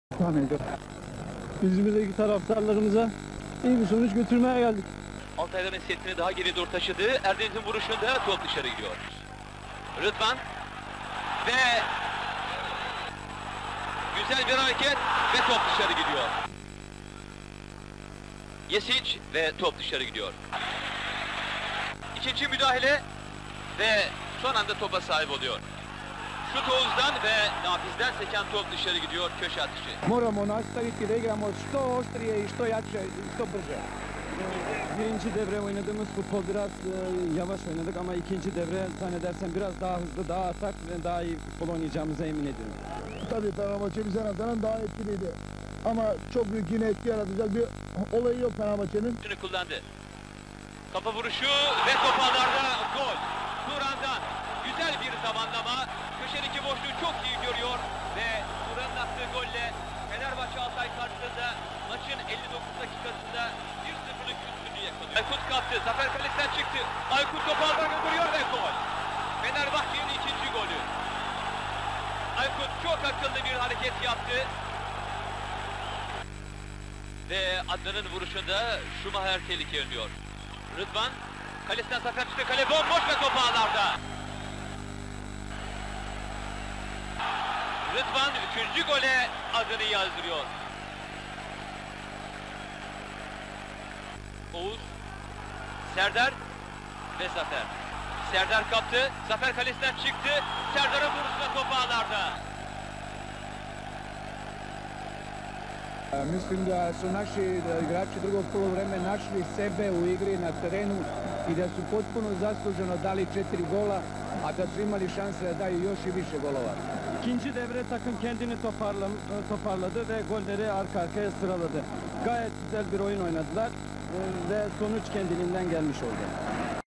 1988 1989 Fenerbahçe Altay KANARYA GOL SHOW YAPTI BETA KAYIDI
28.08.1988
fenerbahçe: toni schumacher, turhan sofuoğlu, taygun erdem, müjdat yetkiner, serdar şenkaya, ergin parlar, oğuz çetin, aykut kocaman (dk. 83 orhan kapucu), şenol çorlu, rıdvan dilmen, erdi demir (dk. 86 önder çakar)

teknik direktör: todor veselinovic (yugoslavya)

altay: zafer öğer, miodrag jesic, adnan yazıcıoğlu, serdar berkin, nafiz tural, miodrag radovic, yüksel kepoğlu (dk. 70 ramazan torunoğlu), mehmet öztürk, alper timur, burak dilmen, aykut yiğit (dk. 70 şeref emeç)

teknik direktör: nevzat güzelırmak

gol:
(1-0) turhan sofuoğlu dk. 59
(2-0) aykut kocaman dk. 61
(3-0) rıdvan dilmen dk. 78
(4-0) serdar şenkaya dk. 84

sarı kartlar:
aykut kocaman dk. 57 (fenerbahçe)
zafer öğer dk. 38, miodrag jesic dk. 44, yüksel kepoğlu dk. 59, burak dilmen dk. 86 (altay)